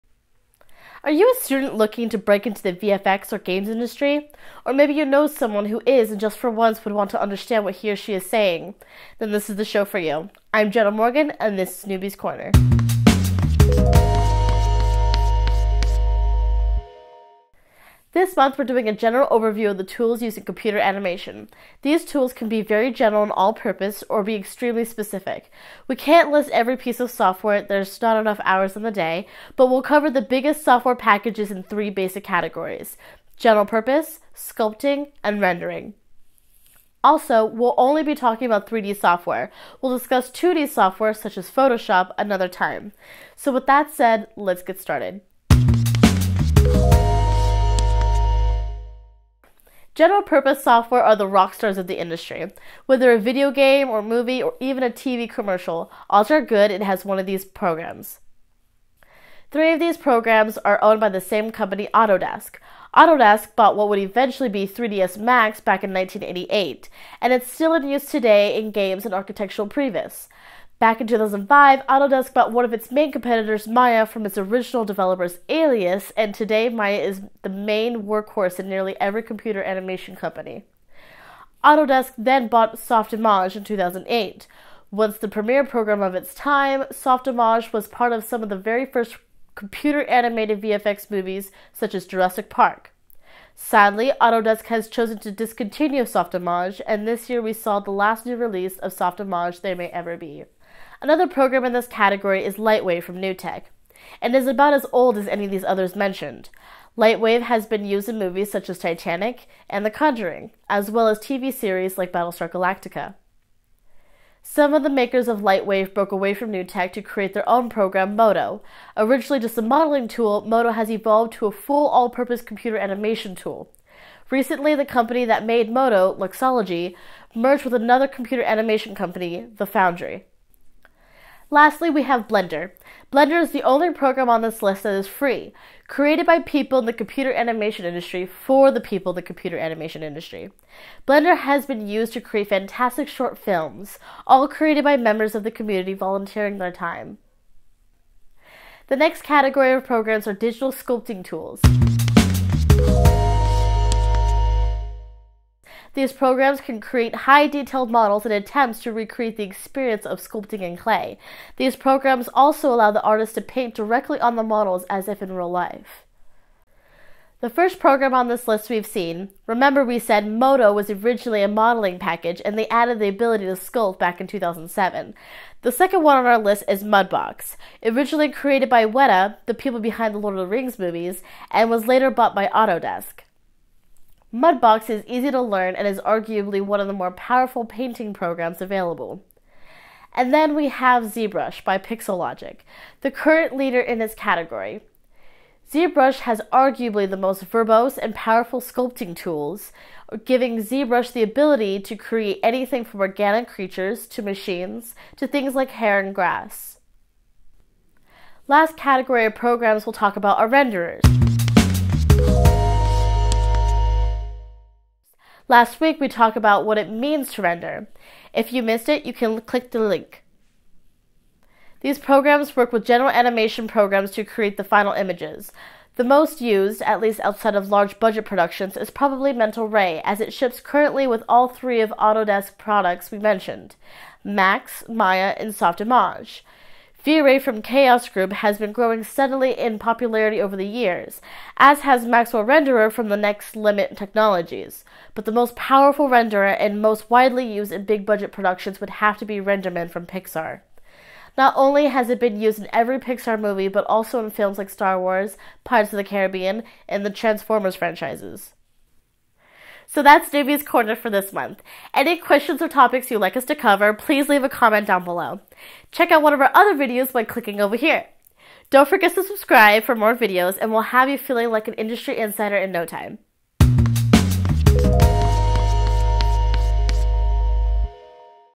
What do they use to make those cool movies or games? Find out in this month's Newbies Corner!
Have you been thinking about getting into visual effects or video games, but don't know what tools or programs they use? This month's episode is a general overview of the programs used by today's industry professionals. Feel free to try out one of these programs - many companies offer trial or student licenses for you to learn with.